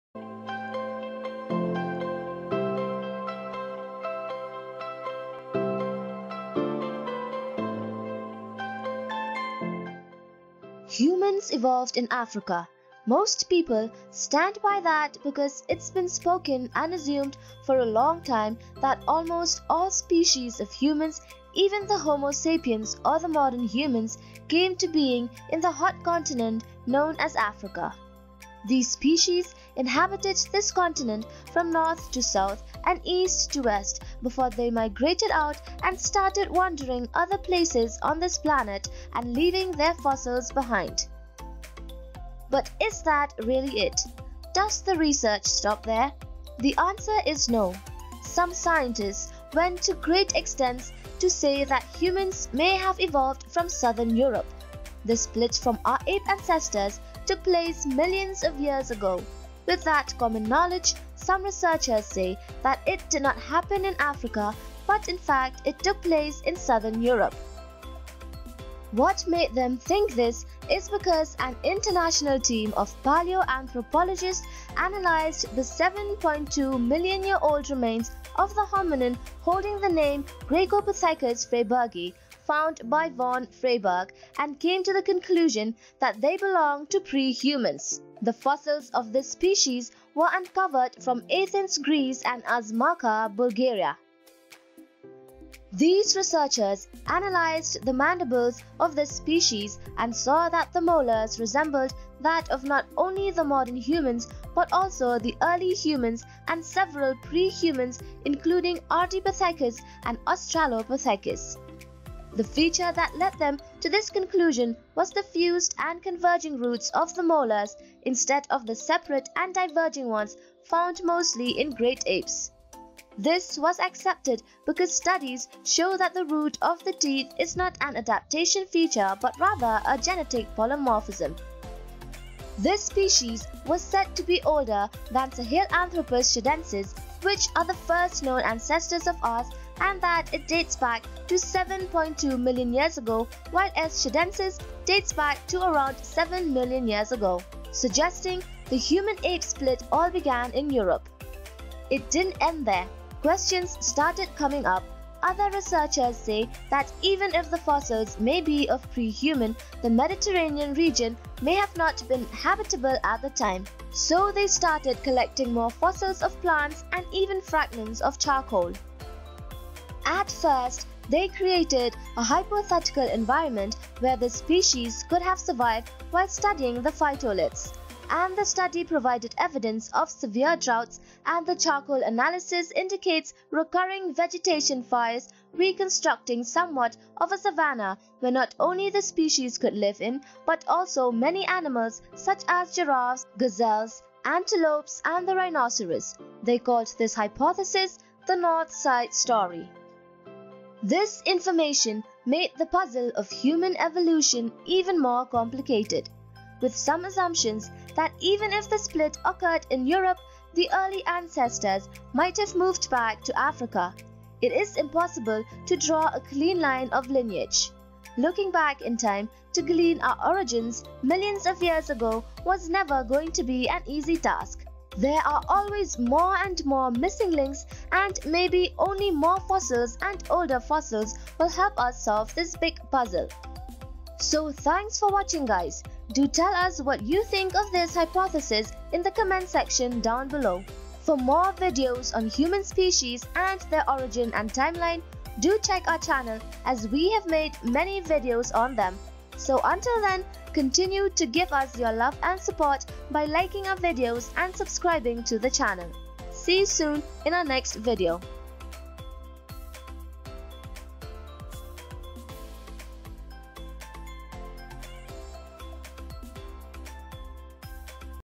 Possible Oldest Human Ancestor From Europe | Graecopithecus freybergi
Humans evolved in Africa. Most people stand by that because it’s been spoken and assumed for a long time that almost all species of humans even the homo sapiens or the modern humans came to being in the hot continent known as Africa. These species inhabited this continent from north to south and east to west before they migrated out and started wandering other places on this planet and leaving their fossils behind.
But is that really it? Does the research stop there? The answer is no. Some scientists went to great extends to say that humans may have evolved from Southern Europe.The split from our ape ancestors took place millions of years ago. With that common knowledge, some researchers say that it did not happen in Africa but in fact it took place in Southern Europe.
What made them think this is because an international team of paleoanthropologists, analyzed the 7.2 million-year-old remains of the hominin holding the name Graecopithecus freybergi found by Von Freyberg and came to the conclusion that they belong to pre-humans. The fossils of this species were uncovered from Athens, Greece and Azmaka, Bulgaria

Halabella does not own the rights to these videos and pictures.
If any content owners would like their images to be given credit,
credit - EPA